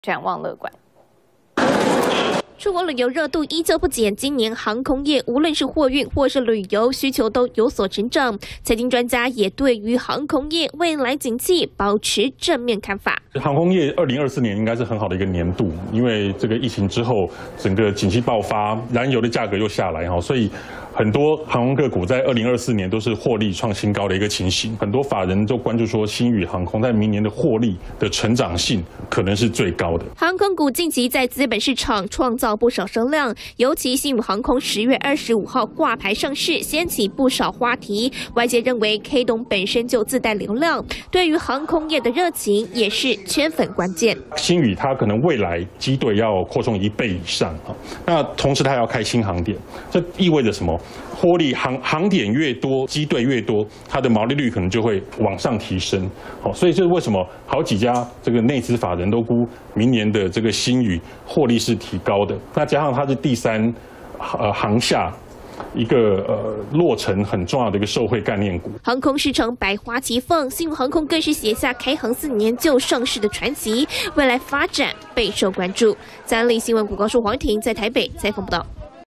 星宇航空上市掀話題！台灣航空四雄爭霸 明年客貨運展望樂觀｜三立新聞網 SETN.com
星宇航空10月25日掛牌上市，掀起市場不少討論，外界除了看好星宇未來獲利空間，對於航空業的未來景氣，也大多持正面看法，認為明年的客運需求將會持續、貨運展望樂觀。